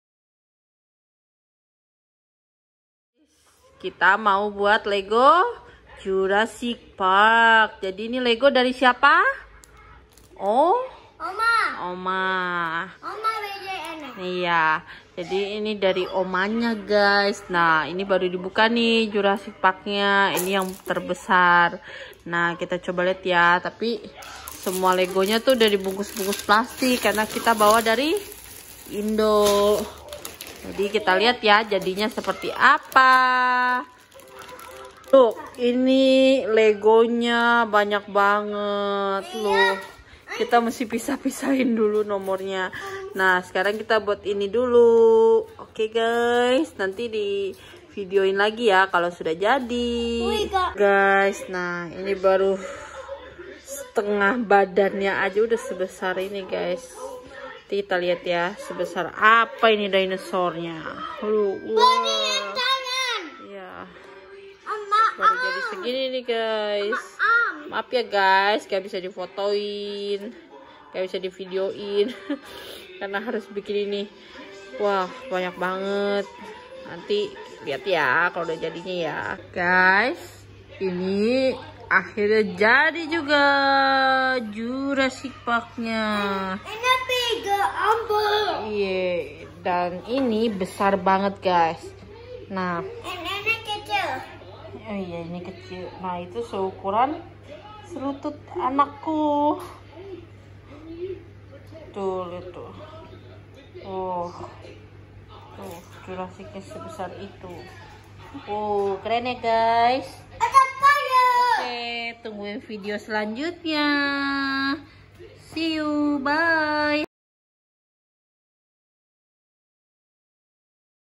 WOW!!!!24 HOURS MAKE THIS BIG LEGO|JURASSIC PARK LEGO|SSJJ FUN
ini lego yg paling besar yang kita buat kebetulan dibeliin sama omanya karna anaknya suka dino dan lego

SSJJ FUN

halo guys. tungguin video terbaru ya dari ssjj fun.

camera :iPhone XR , KB X250